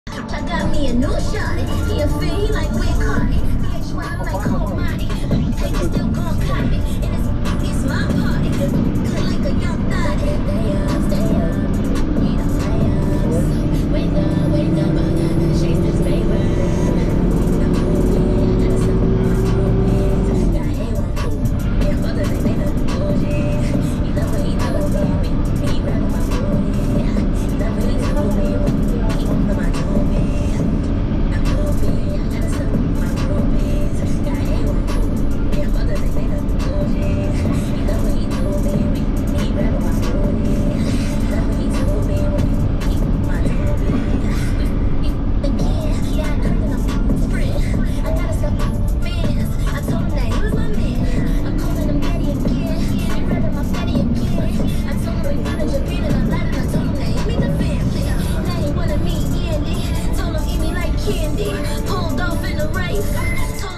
Riding a bus in Nassau, Bahamas
Going from Cable Beach to Downtown